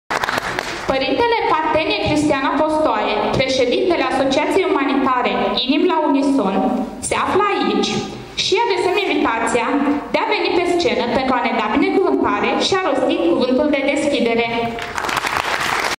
1
Festivalul egalității șanselor "Inimi la unison", Botoșani, 10 mai 2014